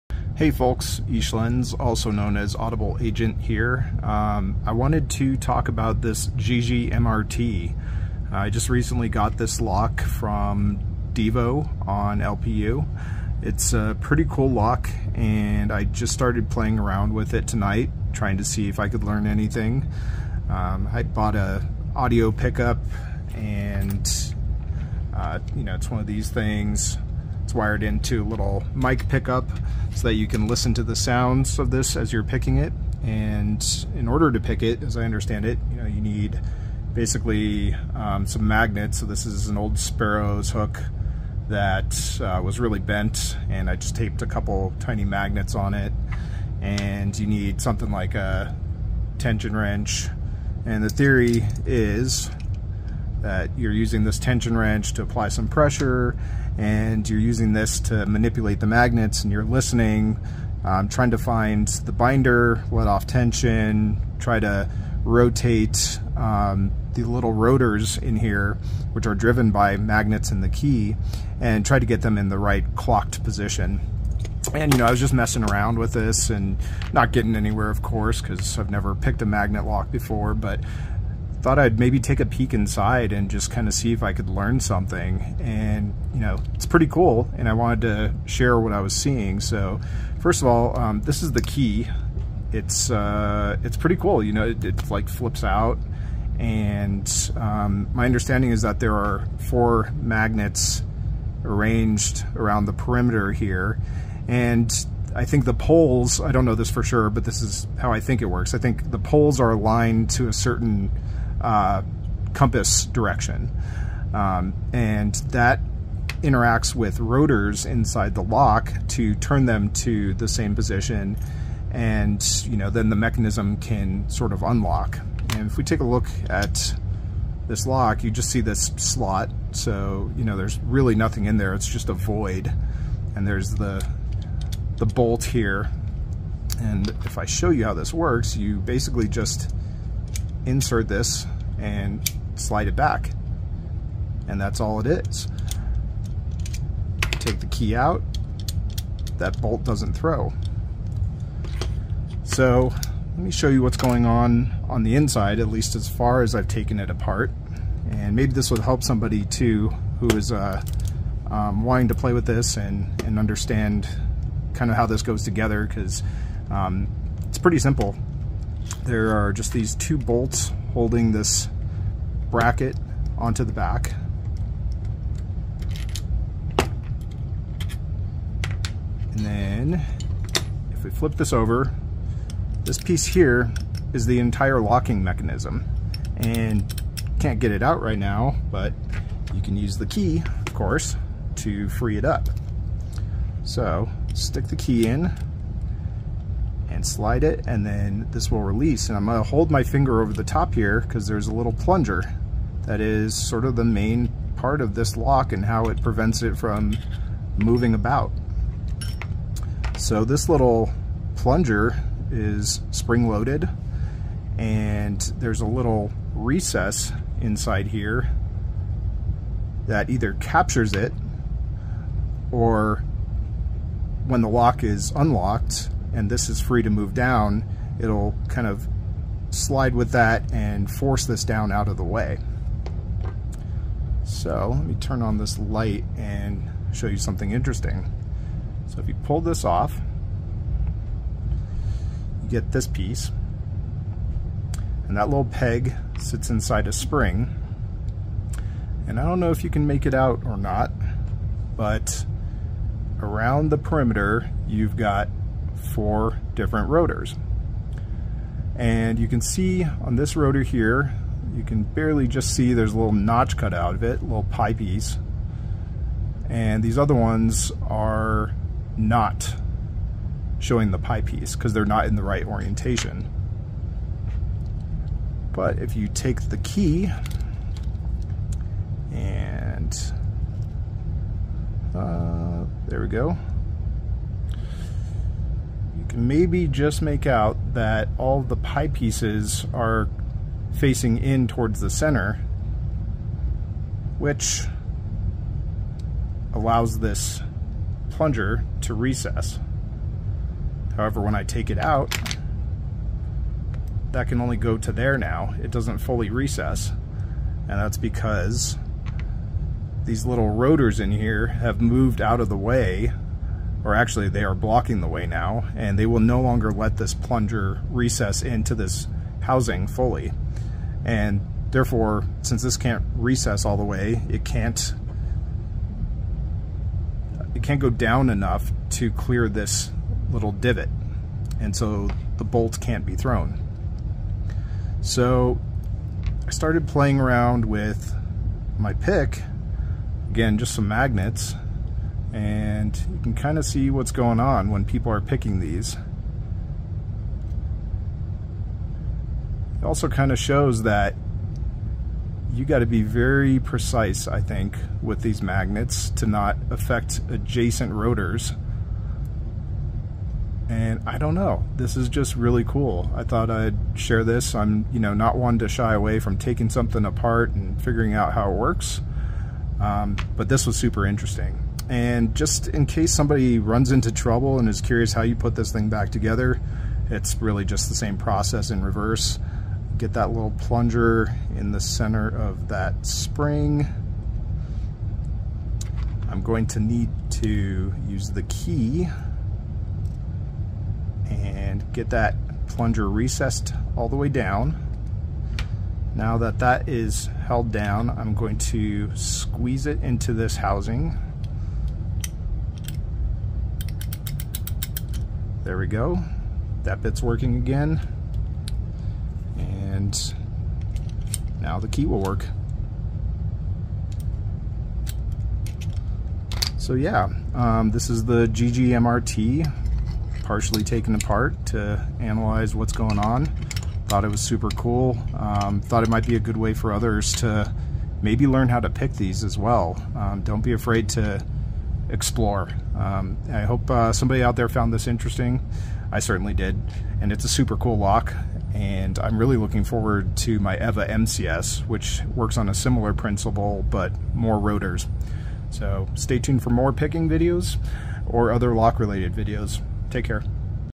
Magnet Lock - GEGE MRT - How It Works
A quick look inside how the GEGE MRT lock works. It's such a neat little mechanism that I had to share. And I think showing how easy it is to take apart could show others how we might be able to learn more about picking these locks. It has already been done, several times, by great lock pickers. But if you're just getting into magnet locks, knowing exactly how it works and how your pick interacts with it is vital information.

I had the choice of several different numbered units. I chose 33 (Avs fan, Patrick Roy)